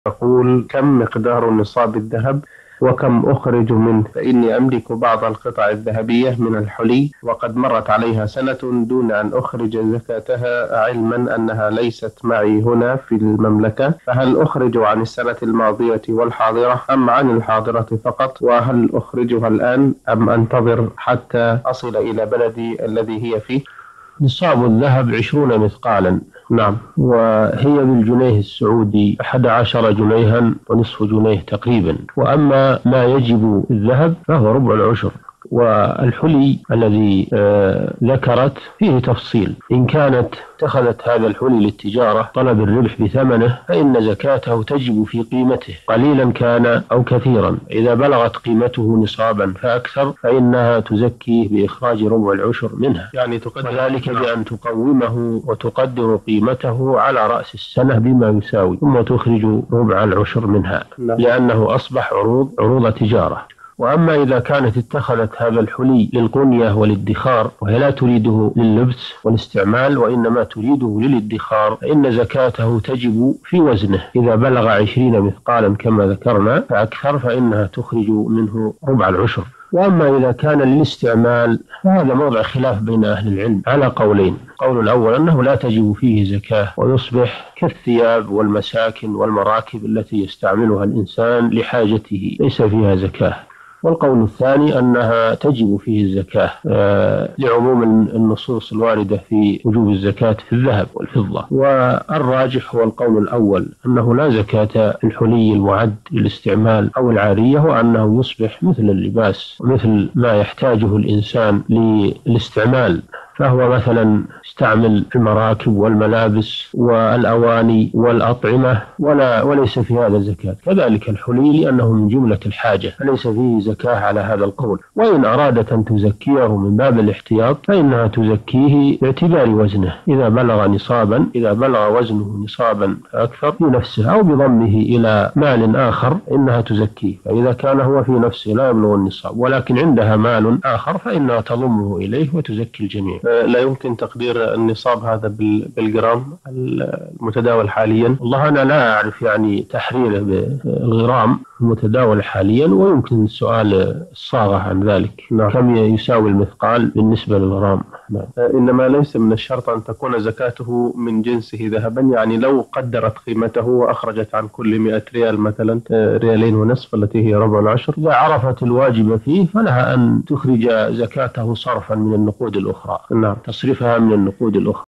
كم مقدار نصاب الذهب وكم أخرج منه وهل أخرج زكاته في المملكة علماً أنه ليس معي في المملكة؟ الفوزان
السيرة الذاتية :
للاشتراك في قناة الشيخ صالح الفوزان